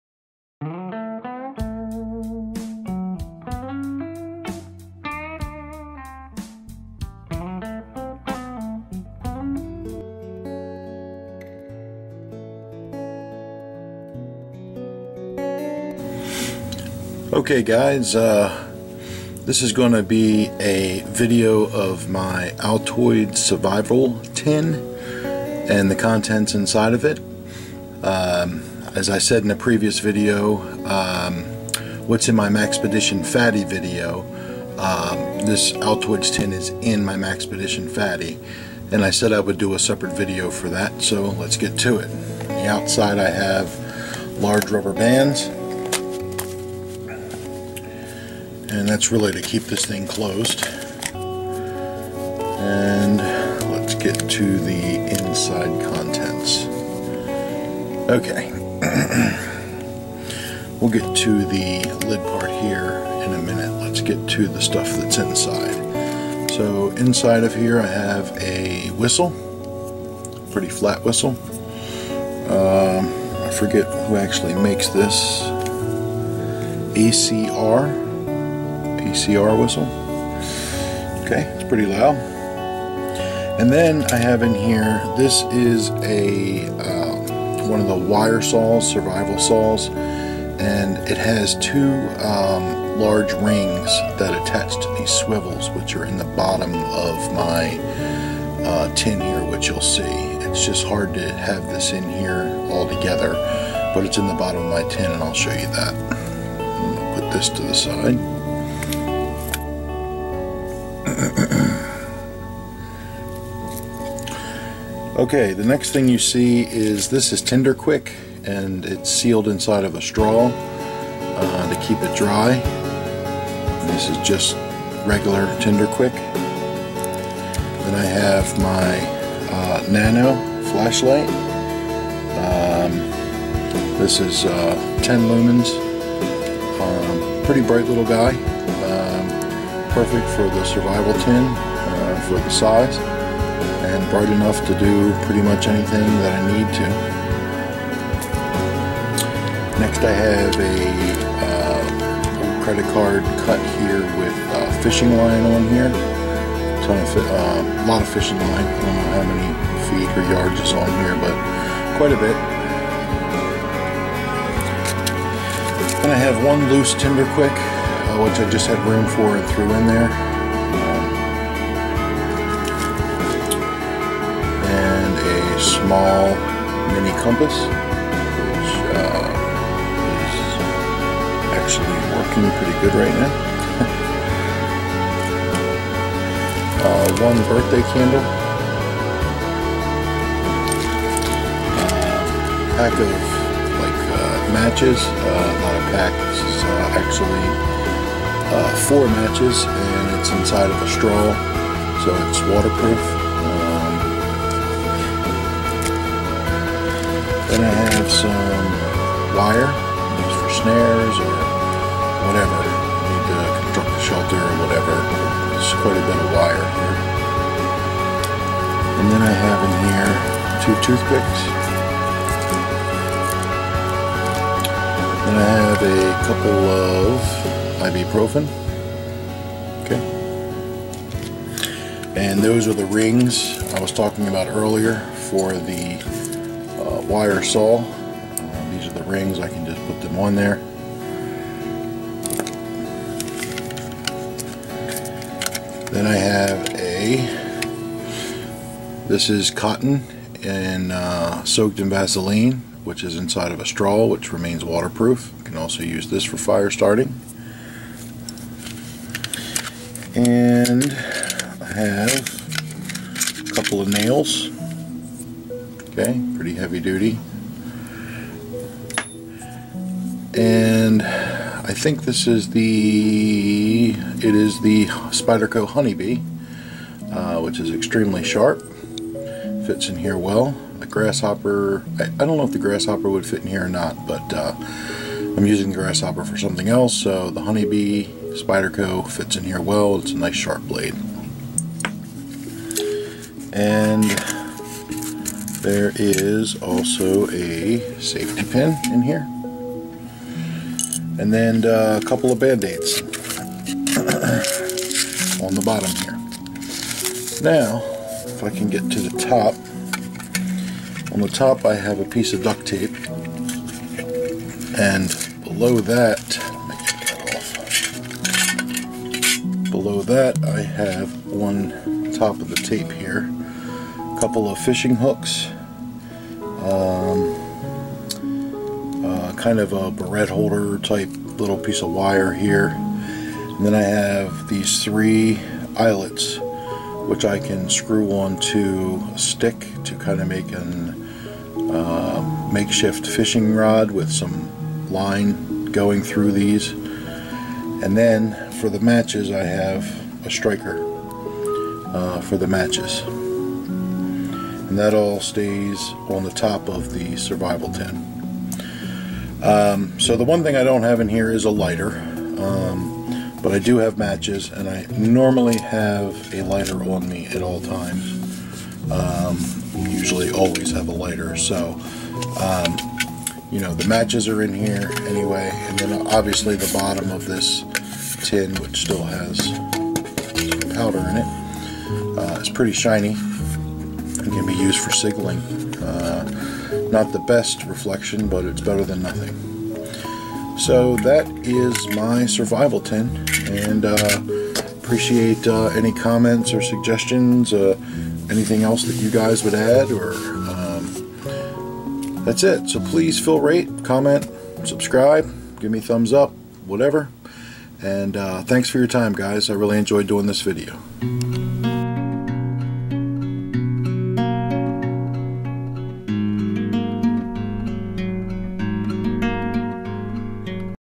Altoids survival tin
The contents in my Altoids survival tin. I now have water purification tablets, and tin foil added to my kit.